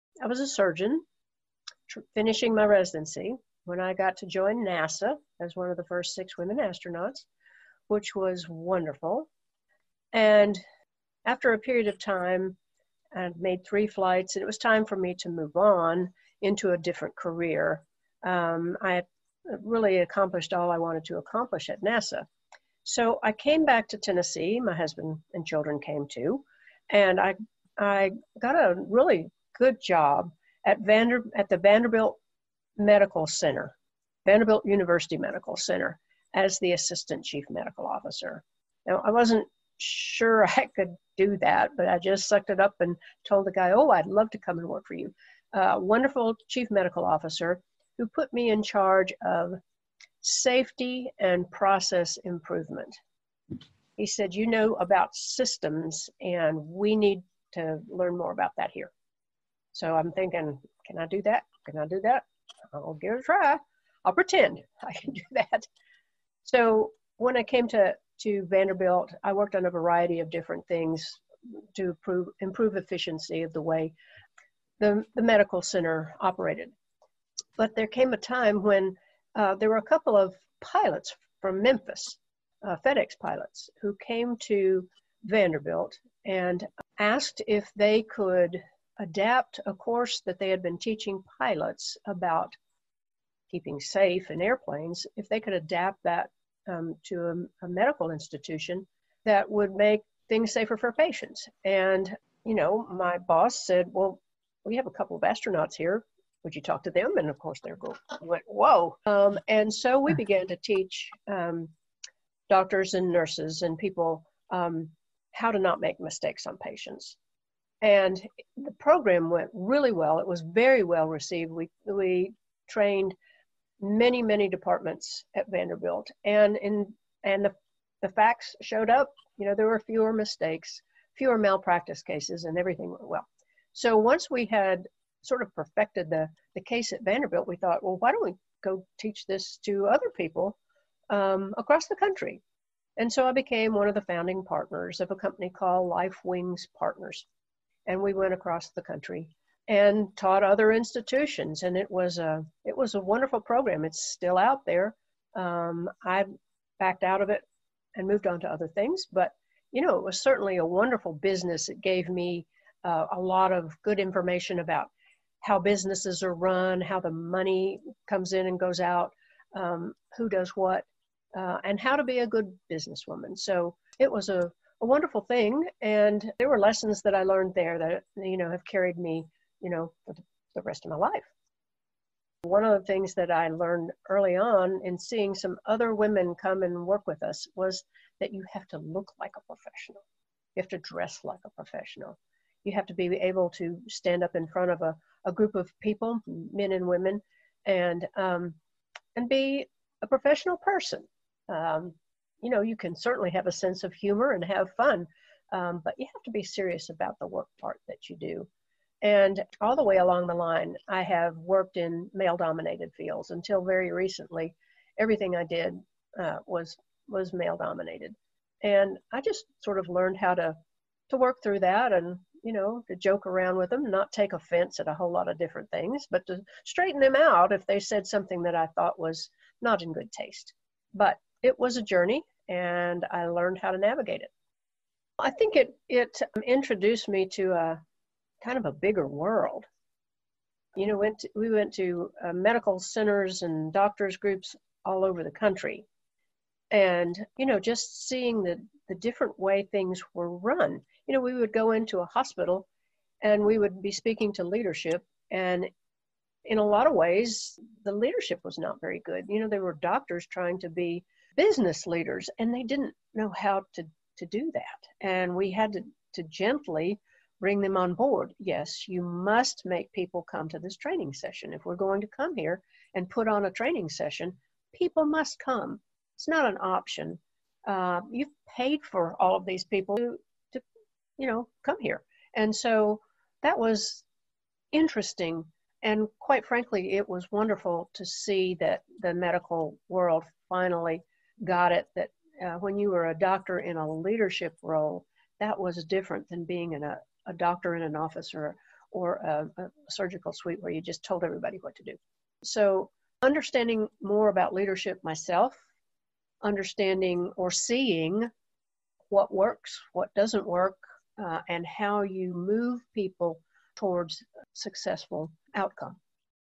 Astronaut Rhea Seddon: Surgeon, Astronaut, Mother, Entrepreneur, Business Owner, Author
In this Better Brew Podcast interview with Host Stephanie Huffman, Astronaut Rhea shares a part of her life few know about. Starting out as a surgeon, she then found herself at NASA. She married a fellow astronaut, became a mother, and after exiting the Space Shuttle program entered the corporate world. She then was a co-founder of a business, and later became an author. In this clip from the interview Dr. Seddon shares her journey as an entrepreneur and some of the adventures along the way.